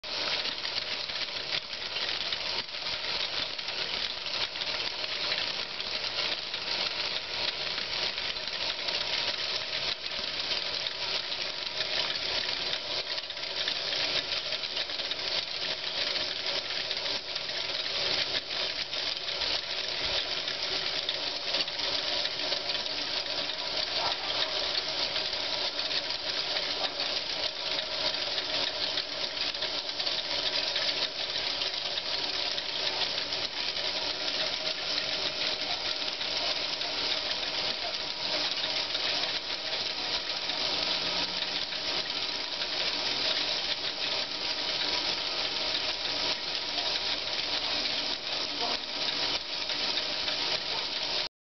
MVI 8120 - Figure-of-8 Meccano Braiding Machine with modified ferrule and increased speed
The overhead ferrule has been improved and the speed of braiding increased in this movie.   The braid from this machine is the tightest from any of my braiders - the white core threads are now enveloped wholly within the braid formed by the circulating bobbin threads.